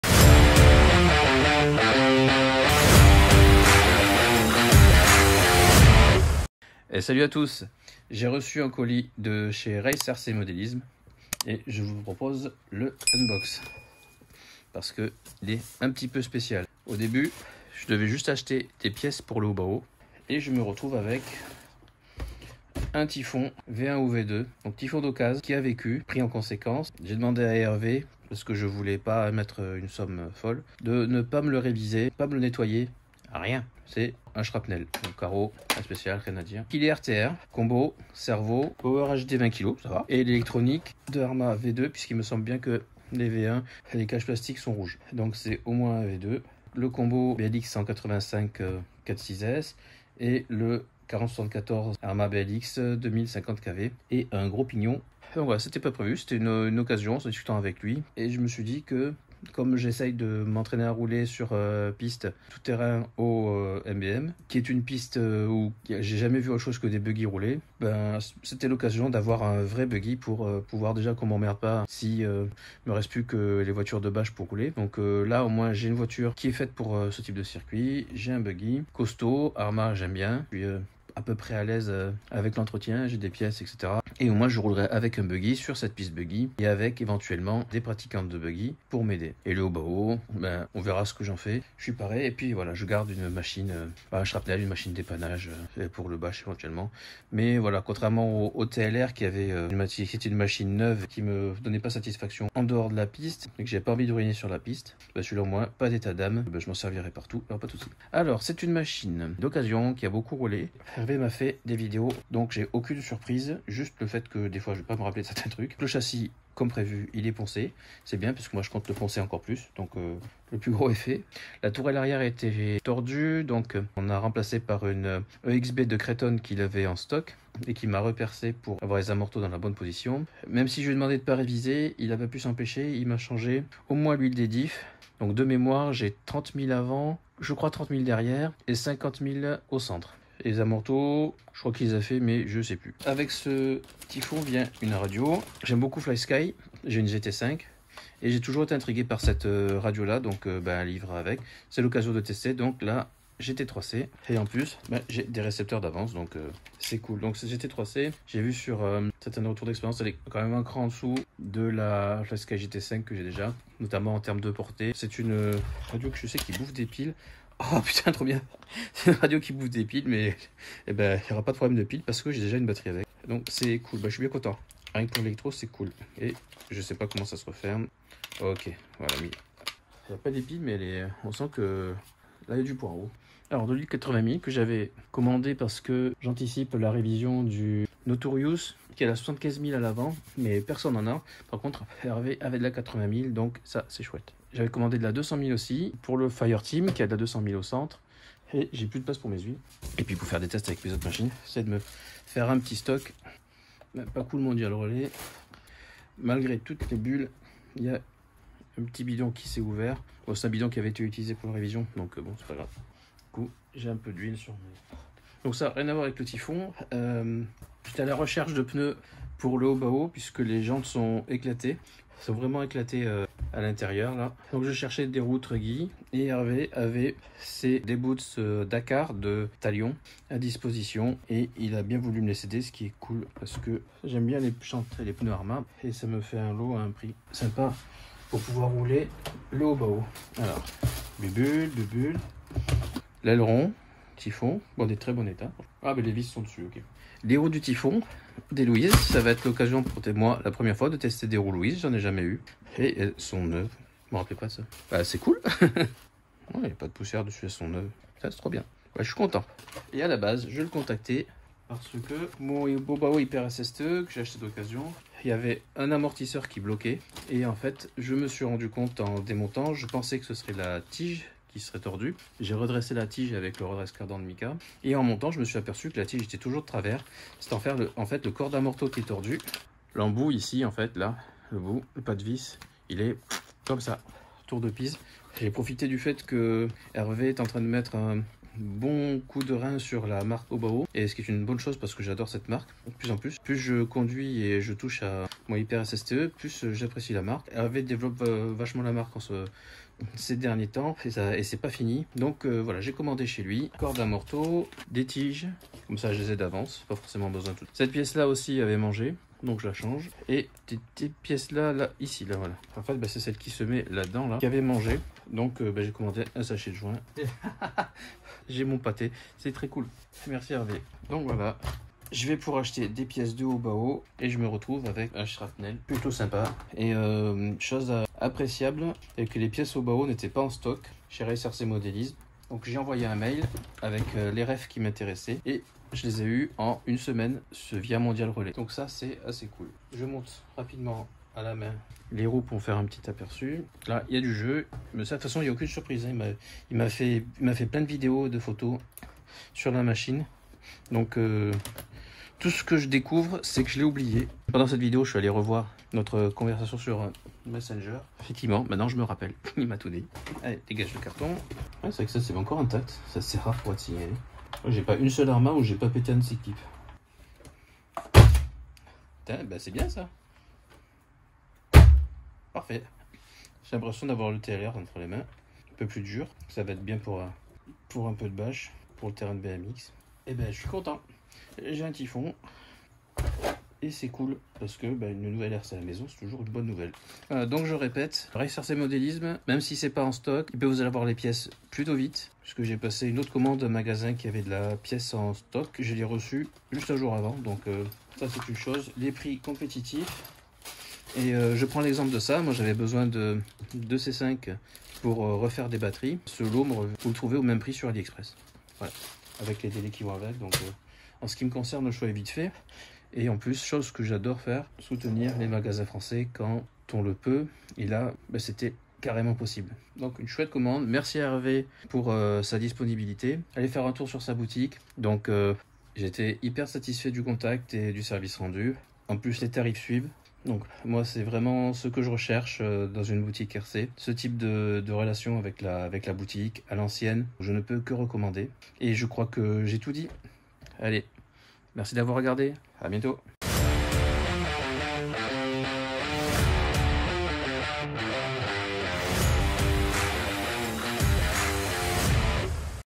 Une Nouvelle RC et des pièces ! Colis de Race RC Modélisme
Merci à Hervé de Race RC Modélisme pour sa bonne humeur et sa passion !

Pour soutenir la chaîne :
👍 like la vidéo
🔔 mets la petite cloche
🖍 et le petit commentaire qui fait plaisir !